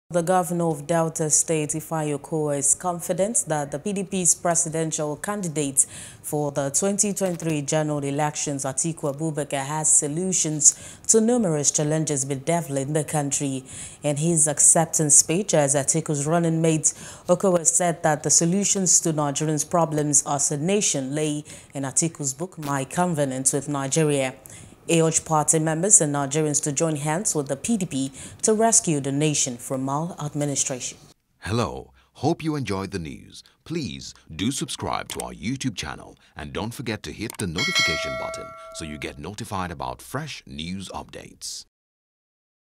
2023 Presidency: Atiku Has Answers To Nigeria's Challenges - Okowa | NEWS
The governor of Delta state, Ifeanyi Okowa, is confident that the PDP’s Presidential Candidate for the 2023 General Elections, Atiku Abubakar, has solutions to the numerous challenges bedeviling the country.